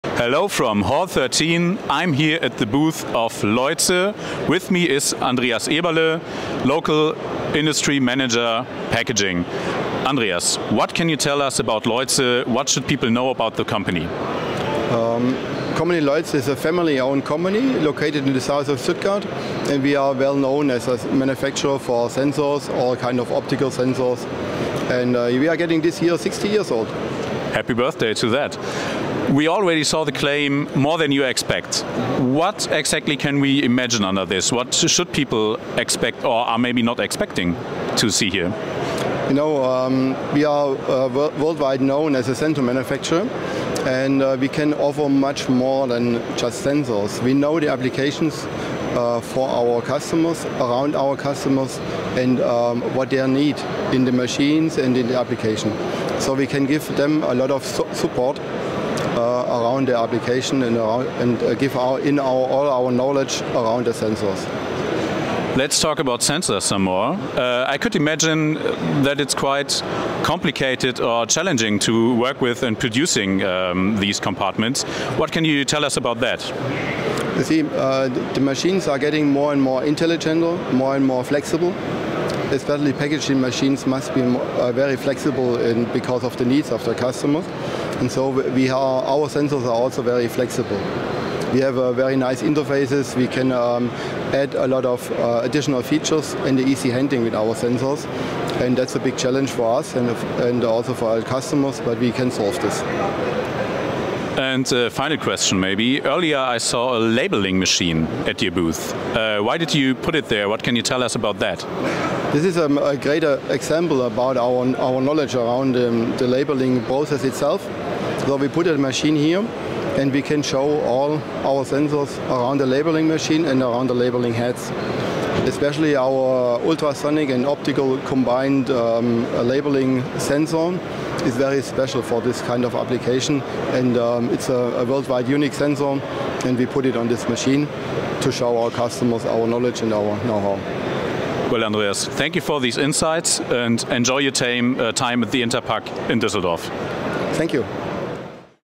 Leuze at interpack 2023
Automated equipment for production and packaging processes must become increasingly flexible, efficient and intelligent. Hygiene and robustness requirements are high in the processing and packaging of food and pharmaceutical products. At interpack 2023, under the motto "More than you expect", Leuze presents how the company supports manufacturers of packaging machines and consumer goods producers in this regard with innovative sensor solutions. From the first to the last step in the packaging process.

Talk with Andreas Eberle, Local Industry Manager Packaging Leuze